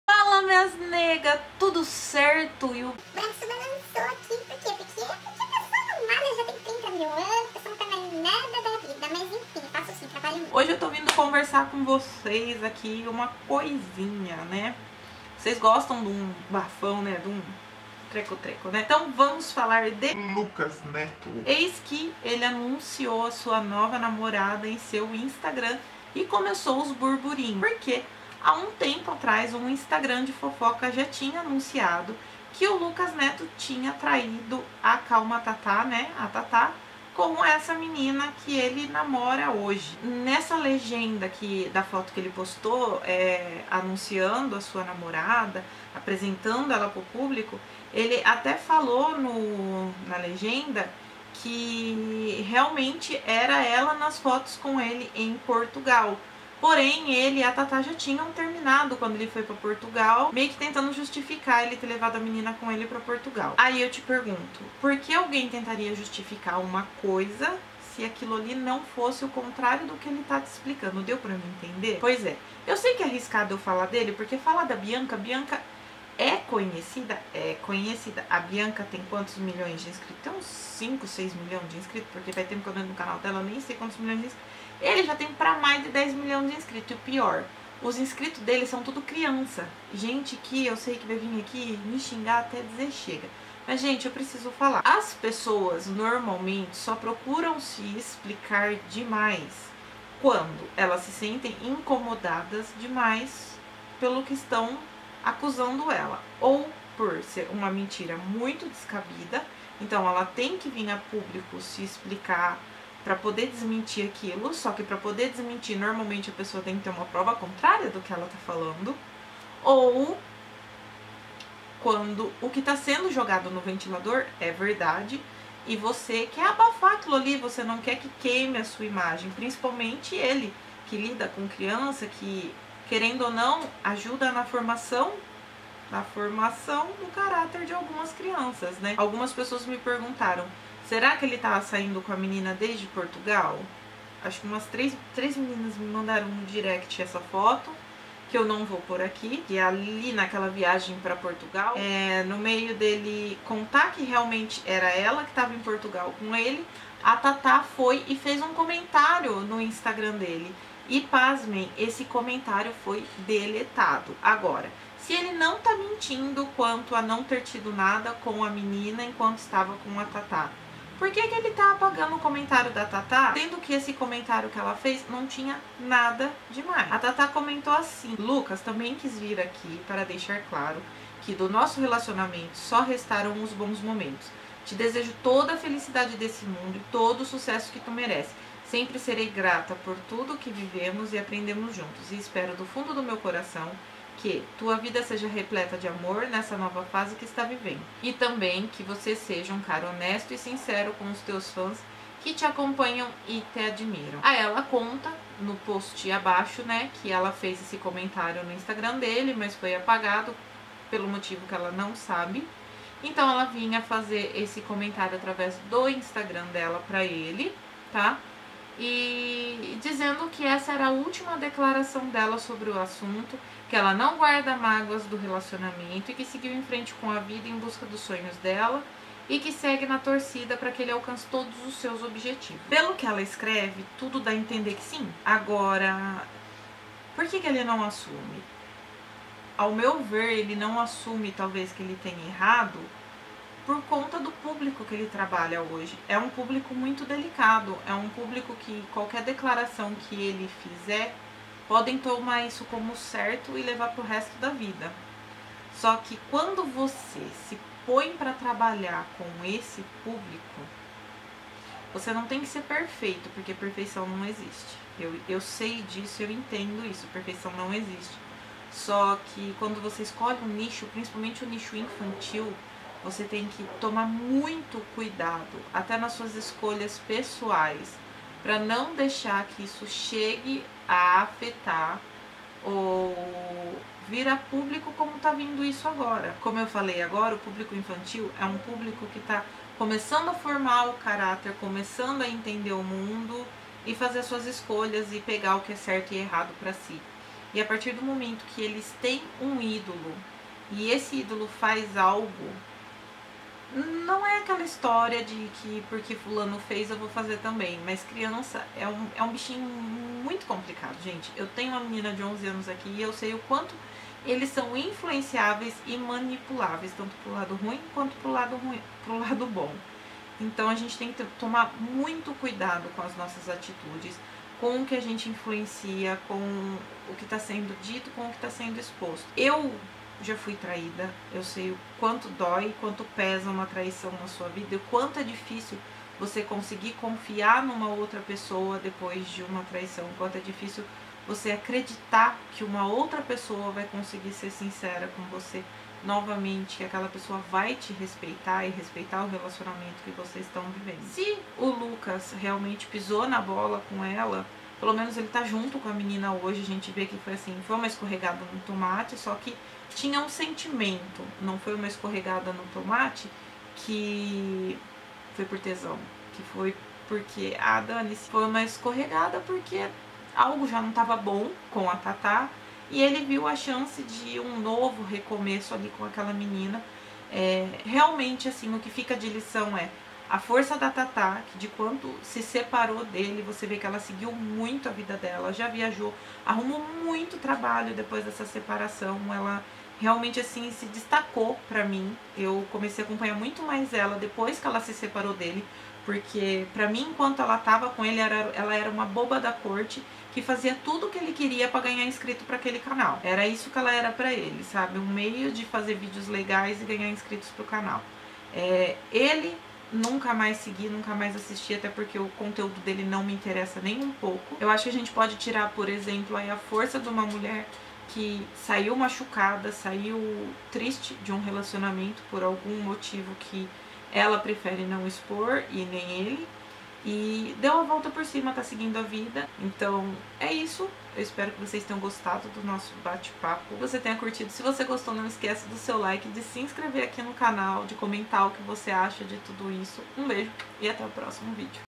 Thata abre o jogo sobre fim com Luccas Neto
ABRA AQUI

Oi negas no falomesmo de hoje vamos falar sobre o fim de Thata e Lucas Neto muitas teorias se formaram mas aos poucos a verdade vai aparecendo!